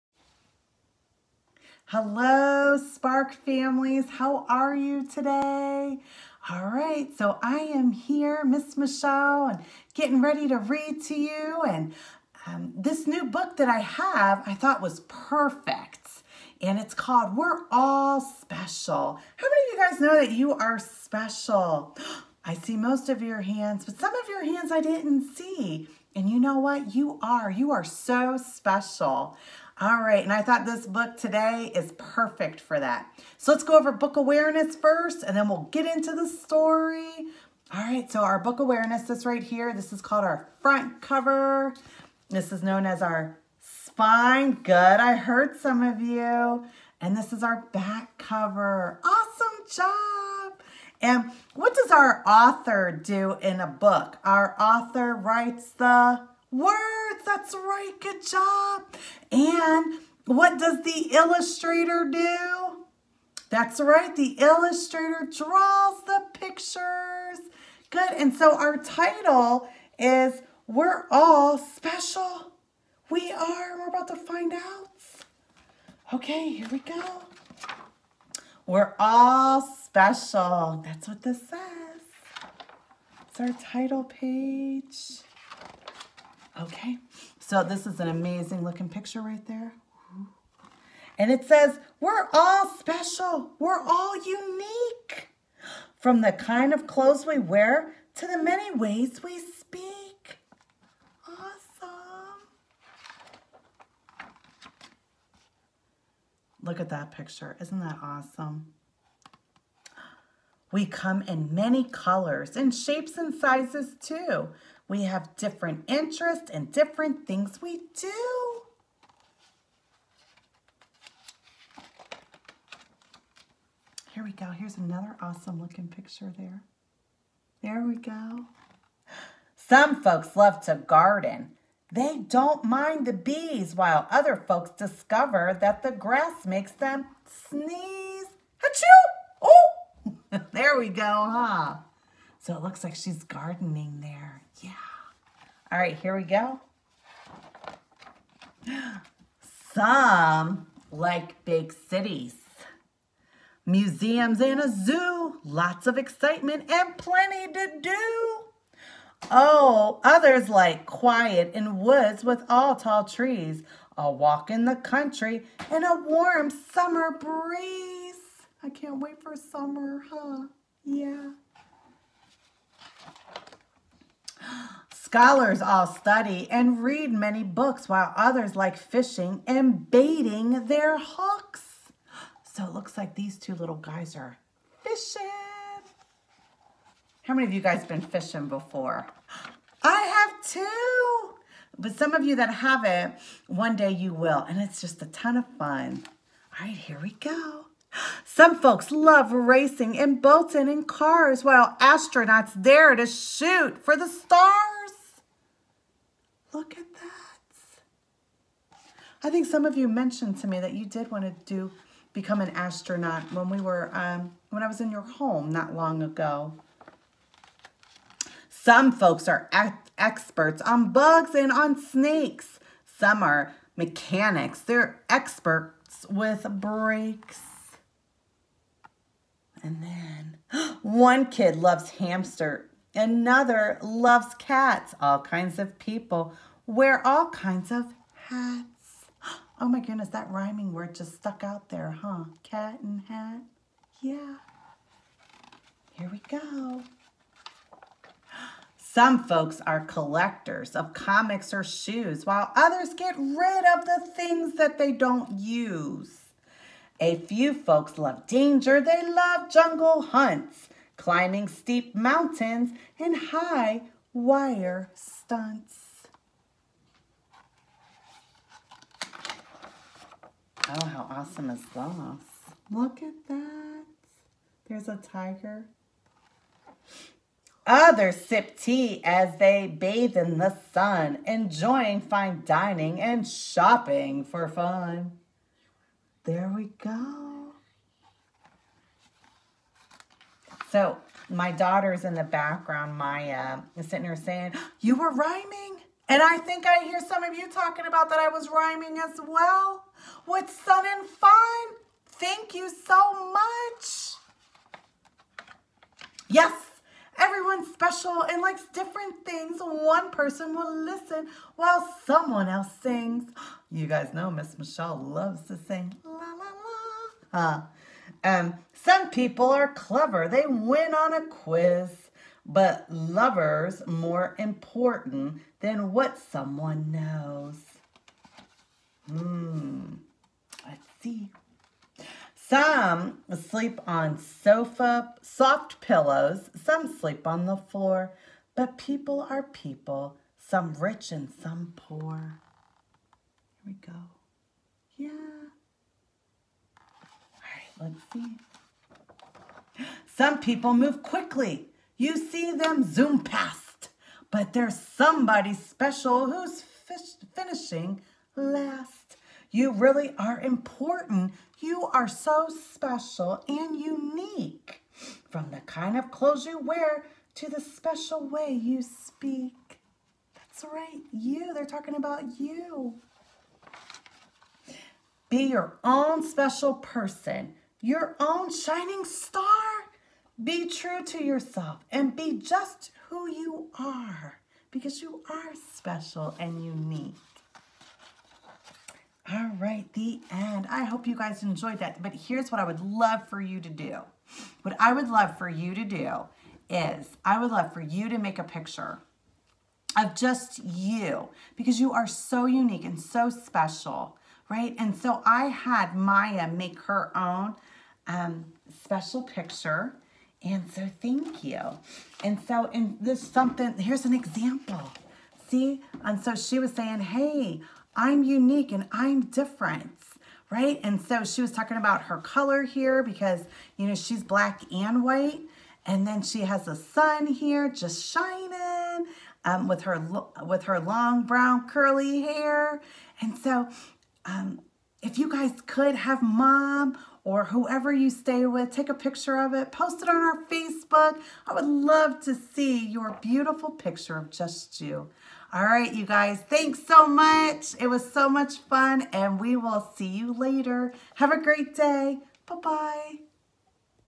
SPARK Ohio Storytime: Episode 13
SPARK Parent Partner, Michelle, reads" We're All Special". Watch til the end as Miss Michelle will make a special request!